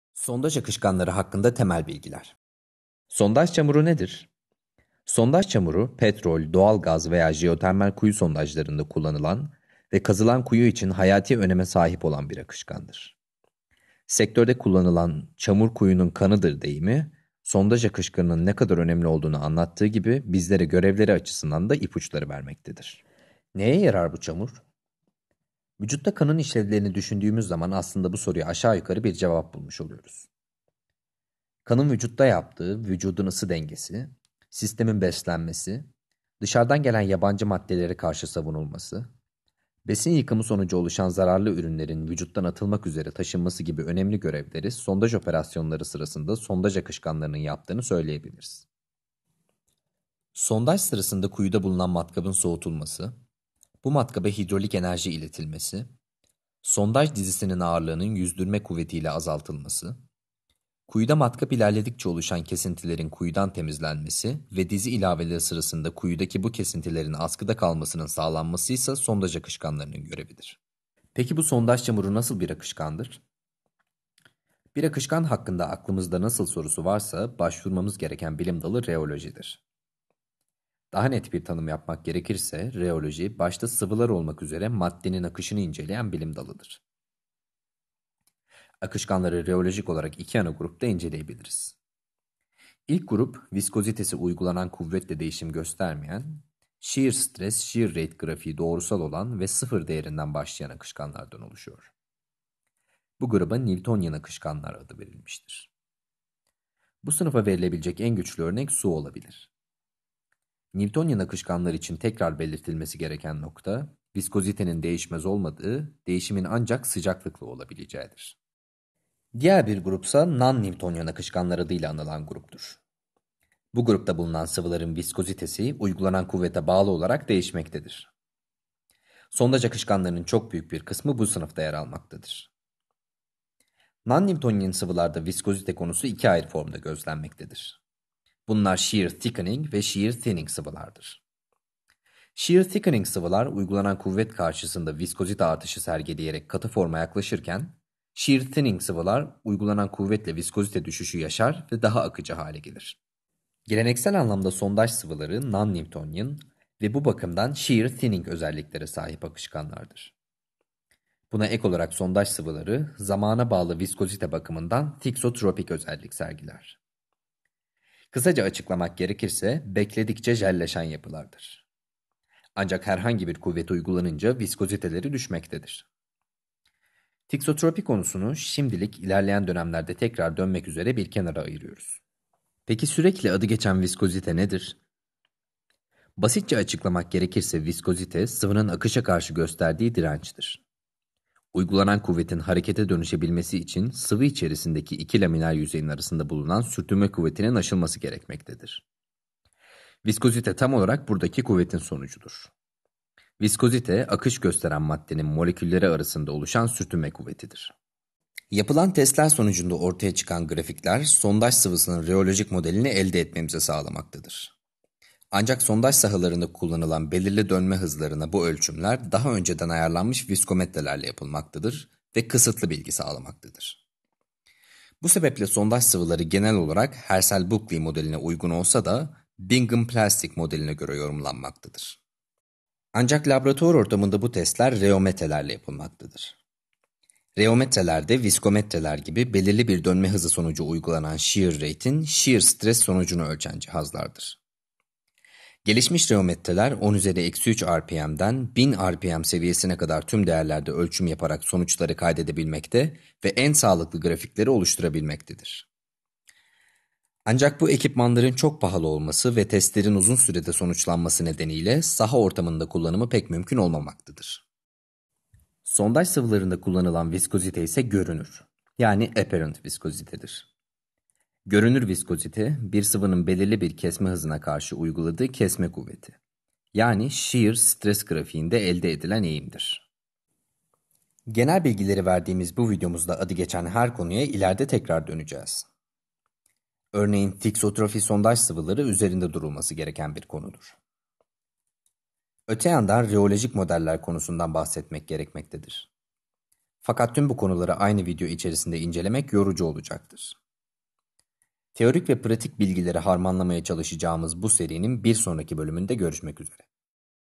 Drilling Fluids - Sondaj Akışkanları (Sondaj Çamuru)
Sondaj akışkanları hakkında temel bilgiler.

Sondaj çamuru nedir?
Sondaj çamuru, petrol, doğalgaz veya jeotermal kuyu sondajlarında kullanılan ve kazılan kuyu için hayati öneme sahip olan akışkandır.
Sektörde kullanılan '' çamur kuyunun kanıdır '' deyimi, sondaj akışkanının ne kadar önemli olduğunu anlattığı gibi, bizlere görevleri açısından da ipuçları vermektedir.

Neye yarar bu çamur? Vücutta kanın işlevlerini düşündüğümüz zaman aslında aşağı yukarı bu soruya cevap bulmuş oluyoruz. Kanın vücutta yaptığı; vücudun ısı dengesi, sistemin beslenmesi, dışarıdan gelen yabancı maddelere karşı savunulması, besin yıkımı sonucu oluşan zararlı ürünlerin vücuttan atılmak üzere taşınması gibi görevleri sondaj operasyonların sondaj akışkanlarının yaptığını söyleyebiliriz. Sondaj sırasında kuyuda bulunan matkabın soğutulması, bu matkaba hidrolik enerji iletilmesi, sondaj dizisinin ağırlığının yüzdürme kuvvetiyle azaltılması, kuyuda matkap ilerledikçe oluşan kesintilerin kuyudan temizlenmesi ve sondaj sırasında dizi ilaveleri sırasında kuyudaki bu kesintilerin askıda kalmasının sağlanması ise sondaj akışkanının görevleridir.

Peki bu sondaj çamuru nasıl bir akışkandır?
Bir akışkanın hakkında aklımızda "nasıl" sorusu varsa başvurmamız gereken bilim dalı reolojidir. Daha net bir tanım yapmak gerekirse; reoloji, başta sıvılar olmak üzere maddenin akışını inceleyen bilim dalıdır.
Akışkanları reolojik olarak iki ana grupta inceleyebiliriz. İlk grup, viskozitesi uygulanan kuvvet ile değişim göstermeyen, shear-stress vs shear-rate grafiği doğrusal olan ve 0 değerinden başlayan akışkanlardan oluşuyor. Bu gruba Newtonian akışkanlar adı verilmiştir. Bu sınıfa verilebilecek en güçlü örnek su olabilir. Newtonian akışkanlar için tekrar belirtilmesi gereken nokta, viskozite değişmez olmadığı ancak değişimin sıcaklık ile olabileceğidir.
Diğer grup ise Non-Newtonian akışkanlar adı ile anılan gruptur. Bu grupta bulunan sıvıların viskozitesi uygulanan kuvvete bağlı olarak değişmektedir. Sondaj akışkanlarının çok büyük bir kısmı bu grupta sınıflandırılmaktadır.
Non-Newtonian sıvılarda viskozite konusu iki ayrı formda gözlenmektedir. Bunlar shear-thickening ve shear-thinning sıvılardır. Shear thickening sıvılar uygulanan kuvvetler karşısında viskozite artışı sergileyerek katı forma yaklaşırken, shear-thinning sıvılar uygulanan kuvvetle viskozite düşüşü yaşar ve daha akıcı bir hale gelir.
Geleneksel anlamda sondaj sıvıları, Non-Newtonian ve bu bakımdan shear-thinning özelliklere sahip akışkanlardır. Buna ek olarak sondaj sıvıları zamana bağlı viskozite bakımından thixotropic özellik sergiler. Kısaca açıklamak gerekirse, bekledikçe jelleşen yapılardır ancak herhangi bir kuvvet uygulanınca viskoziteleri düşmektedir. Thixotropy konusunu ilerleyen dönemlerde tekrar dönmek üzere bir kenara şimdilik ayırıyoruz.

Peki sürekli adı geçen viskozite nedir?
Basitçe açıklamak gerekirse viskozite, sıvının akışa karşı gösterdiği dirençtir. Uygulanan kuvvetin harekete dönüşebilmesi için sıvı içerisindeki iki laminar yüzeyin arasında bulunan sürtünme kuvvetinin aşılması gerekmektedir. Viskozite tam olarak buradaki kuvvetin sonucudur. Viskozite akış gösteren maddenin moleküllerinin arasında oluşan sürtünme kuvvetidir.

Sondaj sıvılarında kullanılan viskozite ise görünür viskozite (apparent viskozite)dir. Görünür viskozite bir sıvının belirli bir kemse hızına karşı uyguladığı kesme kuvveti (shear-stress) grafiğinde elde edilen eğimdir.

Peki bu eğimi bilmek ne işe yarar?

Yapılan testler sonucunda ortaya çıkan grafikler sondaj sıvısının reolojik modelini elde etmemizi sağlamaktadır. Ancak sondaj sahalarında kullanılan belirli (600,300,200,100,6,3) dönme hızlarına daha önceden ayarlanmış viscometrelerle bu ölçüm yapılmaktadır ve kısıtlı bilgi sağlamaktadır. Bu sebeple sondaj sıvıları genel olarak Herschel-Bulkley modeline uygun olsa da Bingham plastic modeline göre yorumlanmaktadır.

Ancak laboratuvar ortamında bu testler reometrelerle yapılmaktadır. Reometreler de viscometreler gibi belirli bir dönme hızı sonucu uygulanan shear-rate'in shear-stress sonucunu ölçen cihazlardır. Gelişmiş reometreler 10-3 rpm'den 1000rpm seviyesine kadar tüm değerlerde ölçüm yaparak sonuçları kaydedebilmekte ve en sağlıklı grafikleri oluşturabilmektedir. Ancak, bu ekipmanların çok pahalı olması ve testlerin uzun sürede sonuçlanması nedeniyle saha ortamında kullanımı pek mümkün olmamaktadır.

Genel bilgileri verdiğimiz bu videomuzda adı geçen her konuya ileride tekrar döneceğiz. Örneğin, thixotropy sondaj sıvıları konusunda üzerinde durulması gereken bir konudur. Öte yandan reolojik modeller konusundan bahsetmek gerekmektedir. Fakat tüm bu konuları aynı video içerisinde incelemek yorucu olacaktır. Teorik bilgiler ve pratik bilgileri harmanlamaya çalışacağımız bu serinin bir sonraki bölümde görüşmek üzere.